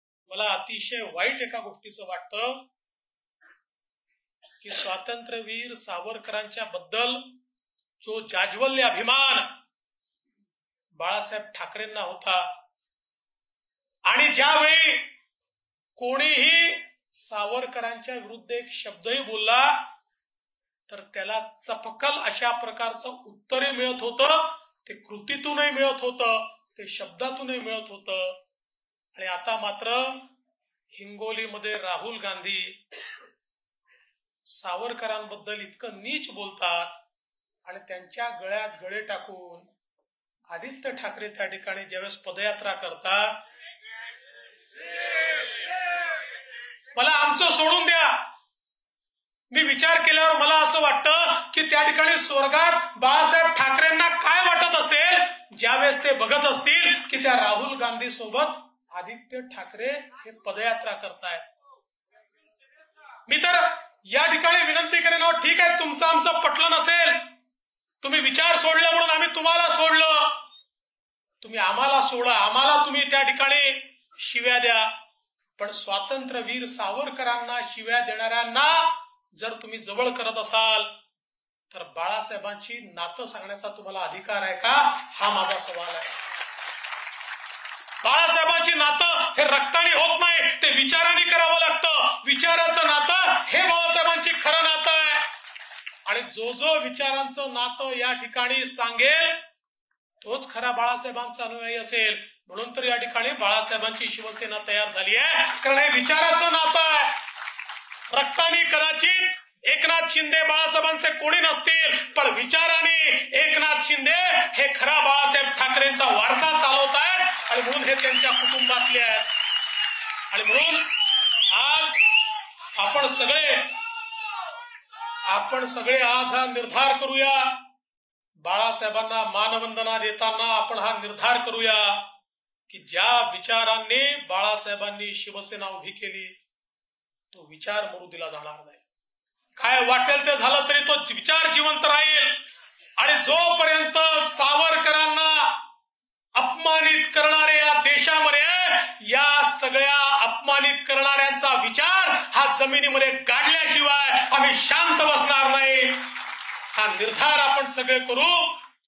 स्व.बाळासाहेबांशी रक्ताने नाही तर विचारांशी नाते असणाराच अनुयायी | मुंबई | DCM Devendra Fadnavis
स्व.बाळासाहेबांशी रक्ताने नाही तर विचारांशी नाते असणाराच अनुयायी | DCM Devendra Fadnavis
('वारसा विचारांचा'. हिंदुत्व - वीर सावरकर ते शिवसेनाप्रमुख बाळासाहेब ठाकरे । मुंबई । १६/११/२२)